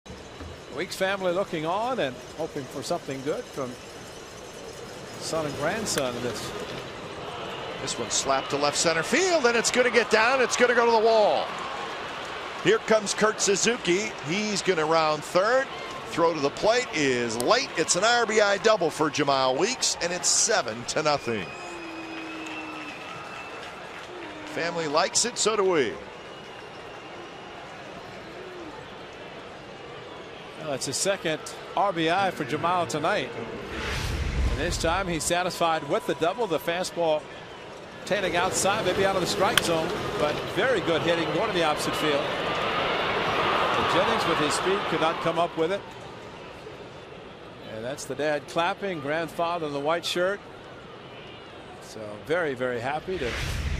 2011/08/06 Weeks' RBI double
OAK@TB: Weeks drives an RBI double into the gap